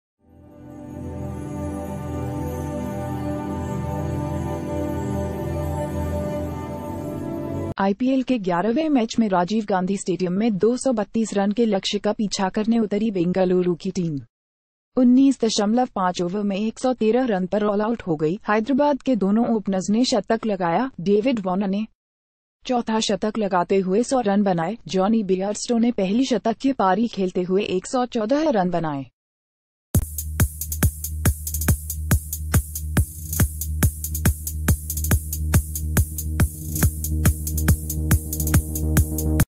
सनराइजर्स हैदराबाद ने रॉयल चैलेंजर्स बेंगलुरु को 118 रन से हराया
आईपीएल के 11वें मैच में राजीव गांधी स्टेडियम में 232 रन के लक्ष्य का पीछा करने उतरी बेंगलुरु की टीम 19.5 ओवर में 113 रन पर ऑल आउट हो गई। हैदराबाद के दोनों ओपनर्स ने शतक लगाया। डेविड वॉर्नर ने चौथा शतक...